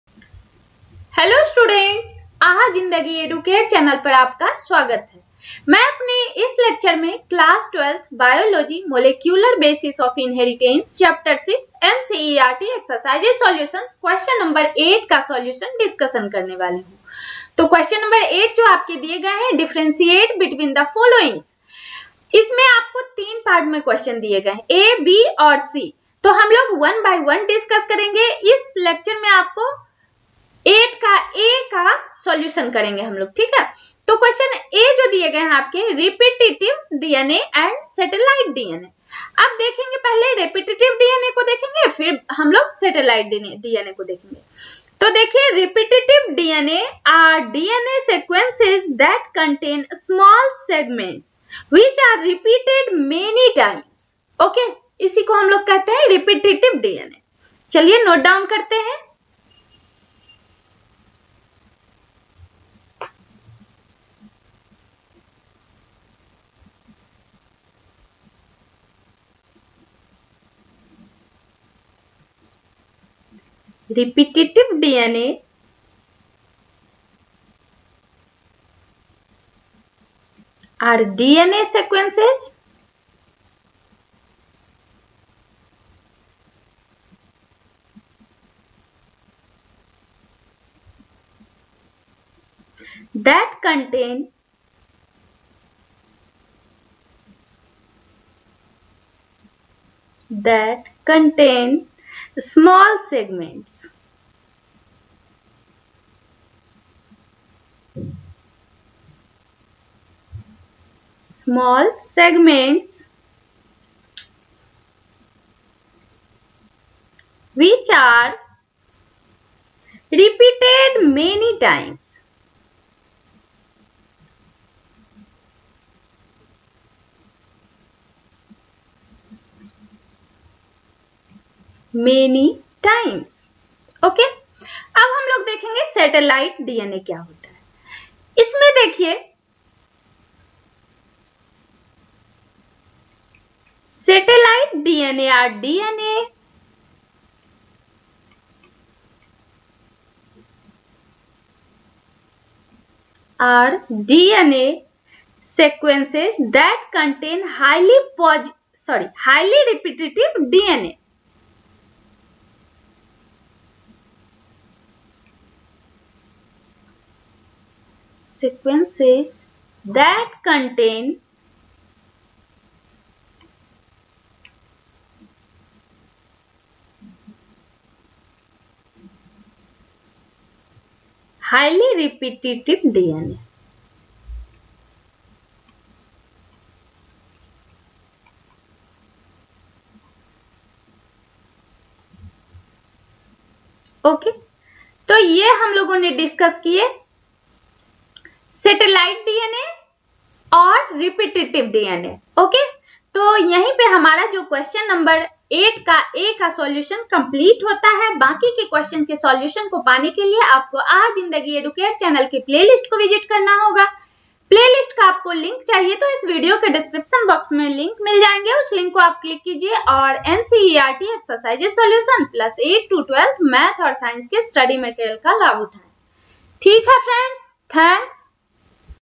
NCERT EXERCISES SOLUTIONS CLASS 12 BIOLOGY chapter-6 (Molecular Basis of Inheritance) Q.8(a)
NCERT EXERCISES SOLUTIONS CLASS 12 BIOLOGY chapter-6 (Molecular Basis of Inheritance) Question Number 8 (a)

important question of molecular basis of inheritance for board,